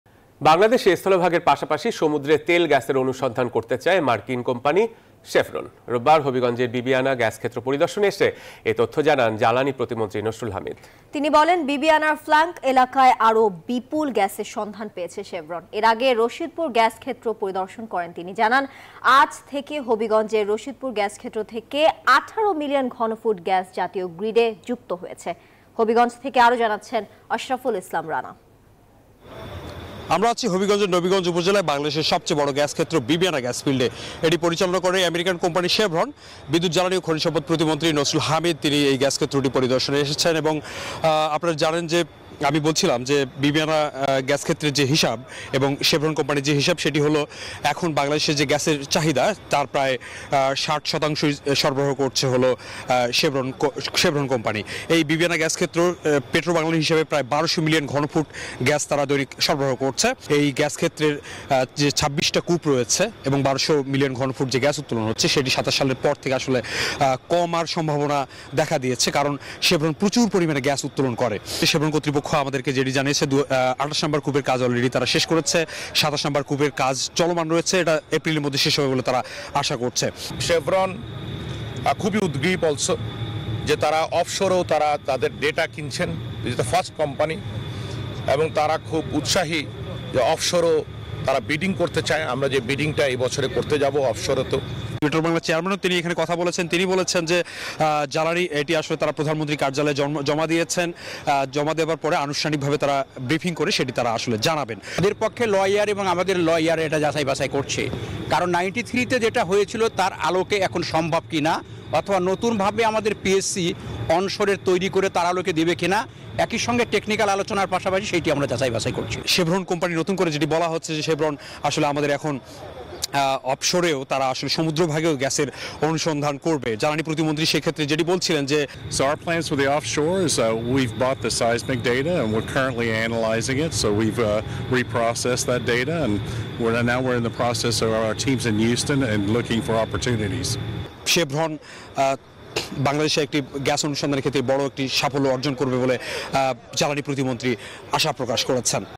বাংলাদেশের সমুদ্রে তেল গ্যাসের অনুসন্ধান করতে চায় মার্কিন কোম্পানি | News24
NEWS24 Team